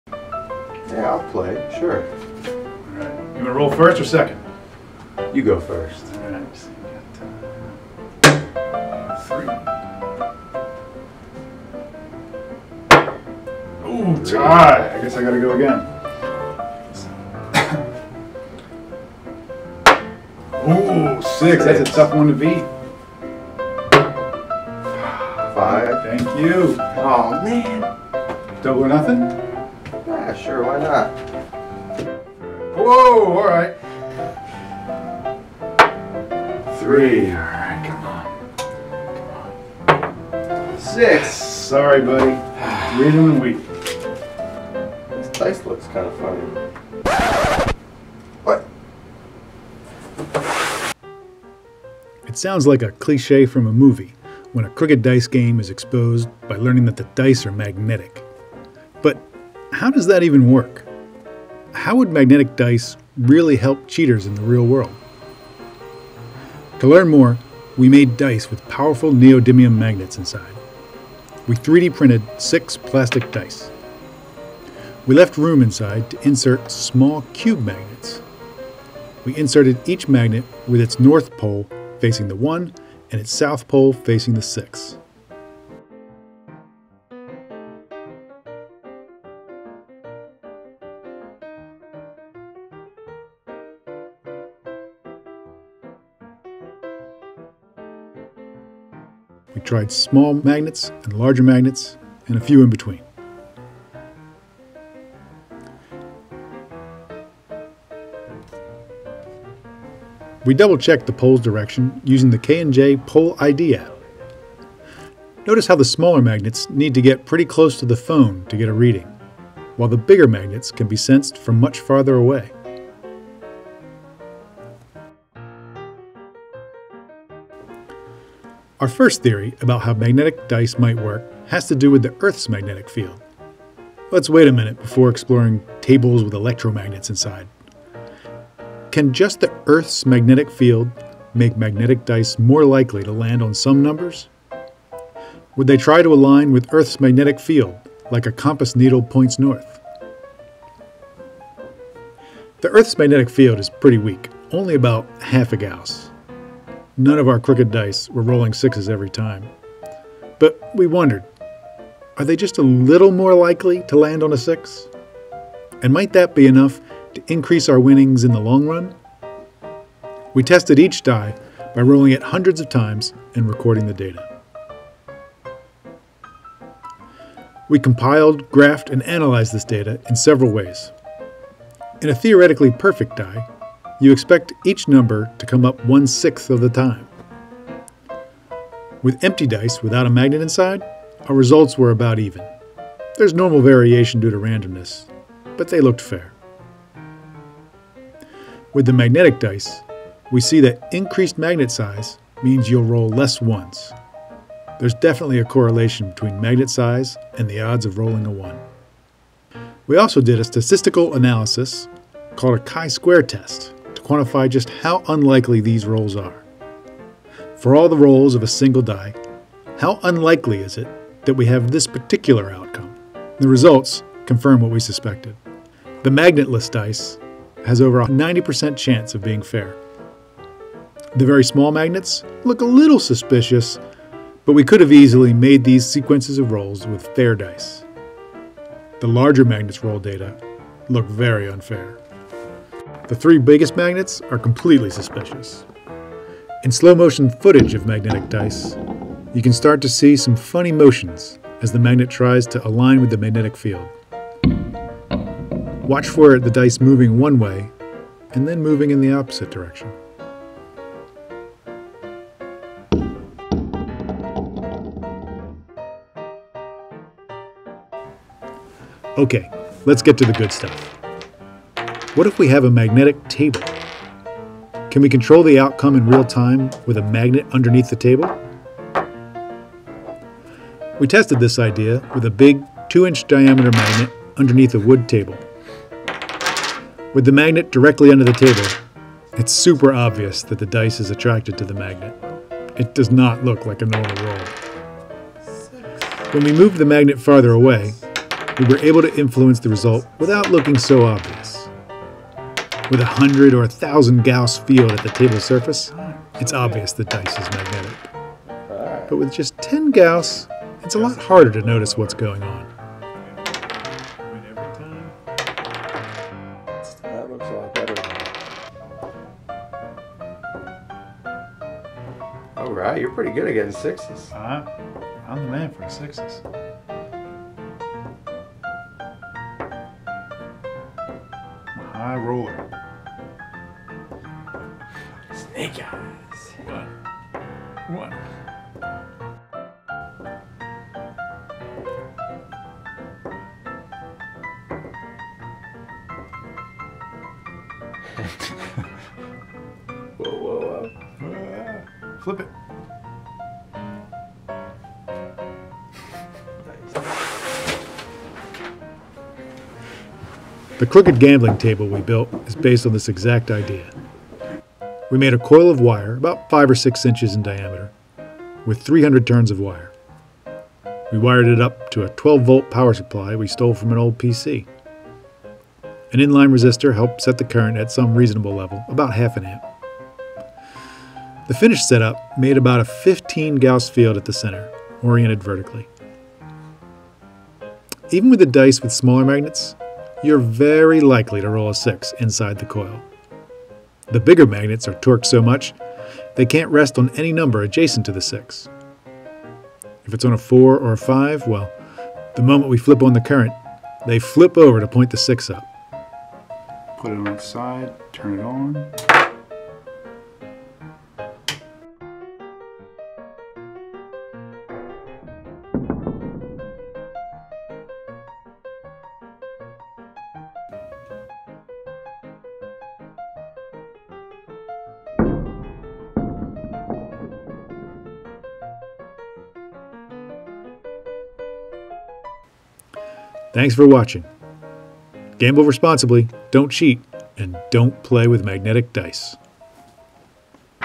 Cheating (Magnetic) Dice, EXPOSED!!
Cheating dice?! With magnets???

Please, do not cheat at dice.  Don’t blame us for getting kicked out of a casino!  We don’t promote or favor making crooked dice.  As always, this exploration was really just an excuse to play with magnets. If you notice your dice stick to steel, though, somebody might be cheating.

From experiments, to DIY projects, to smash videos, we've got it all!